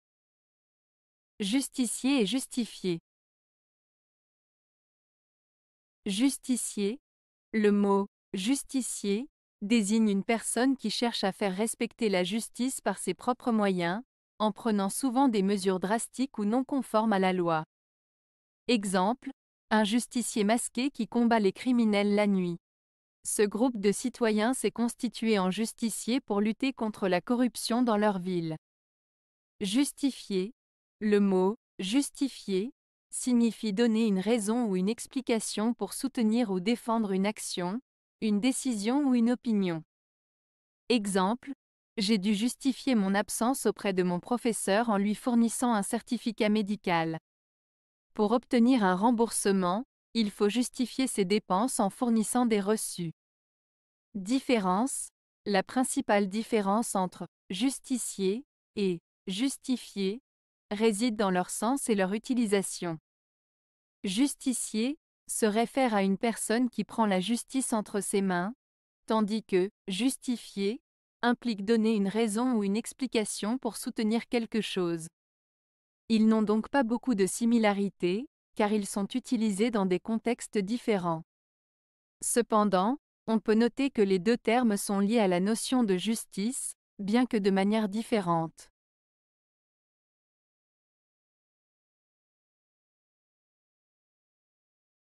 Justicier et Justifier (Une Lettre)
Justicier:
Le mot "justicier" désigne une personne qui cherche à faire respecter la justice par ses propres moyens, en prenant souvent des mesures drastiques ou non conformes à la loi.

Exemples:
- Un justicier masqué qui combat les criminels la nuit.
- Ce groupe de citoyens s'est constitué en justiciers pour lutter contre la corruption dans leur ville.

Justifier:
Le mot "justifier" signifie donner une raison ou une explication pour soutenir ou défendre une action, une décision ou une opinion.

Exemples:
- J'ai dû justifier mon absence auprès de mon professeur en lui fournissant un certificat médical.
- Pour obtenir un remboursement, il faut justifier ses dépenses en fournissant des reçus.

Différence:

La principale différence entre "justicier" et "justifier" réside dans leur sens et leur utilisation. "Justicier" se réfère à une personne qui prend la justice entre ses mains, tandis que "justifier" implique donner une raison ou une explication pour soutenir quelque chose. Ils n'ont donc pas beaucoup de similarités, car ils sont utilisés dans des contextes différents. Cependant, on peut noter que les deux termes sont liés à la notion de justice, bien que de manière différente.